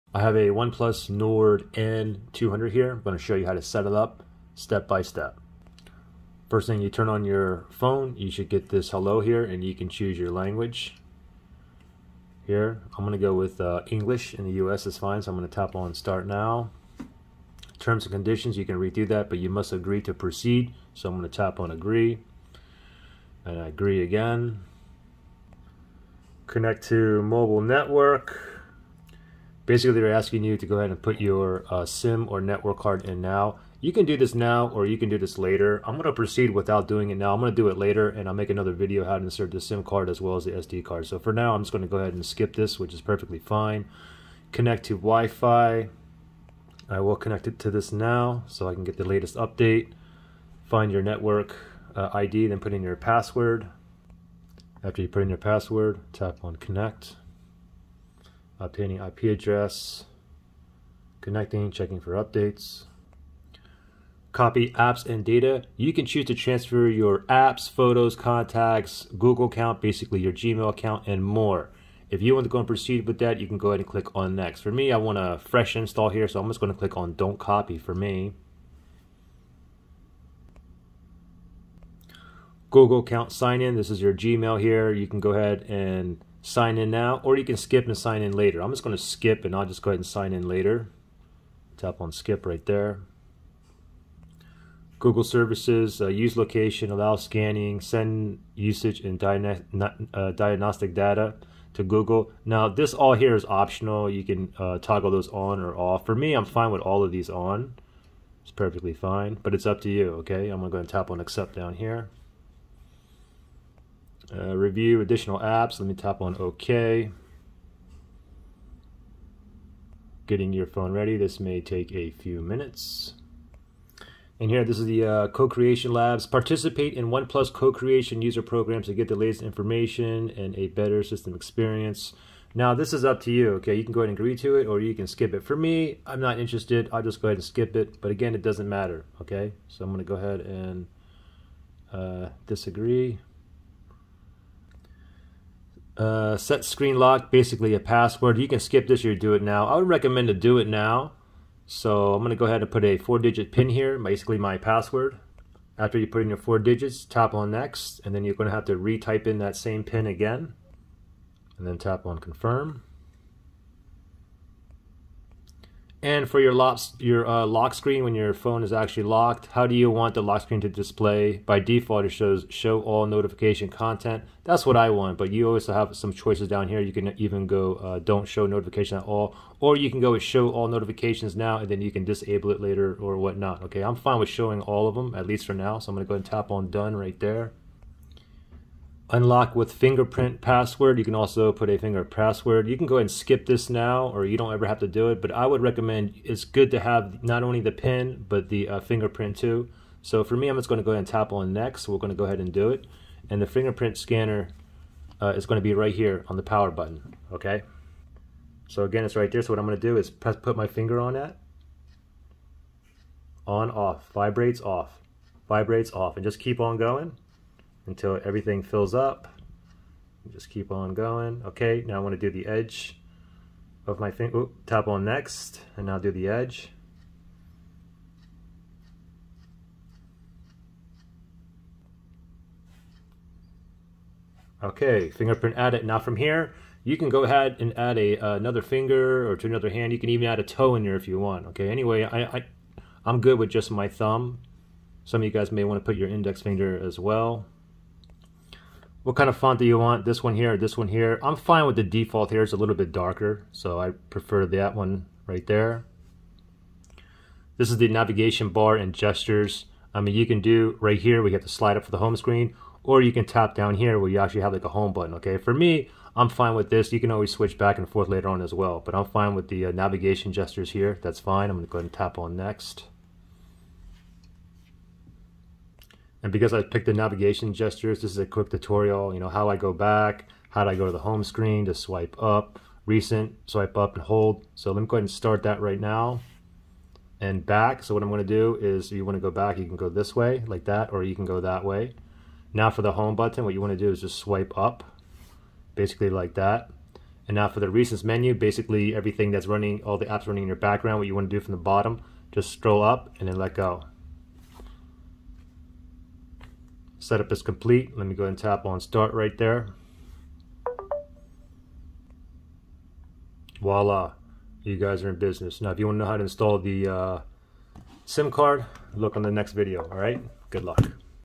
OnePlus Nord N200: How to Setup (step by step for beginners)
I show you how to how to setup (initial setup) the OnePlus Nord N200 phone step by step from the beginning.

OnePlus Nord N200 | 5G Unlocked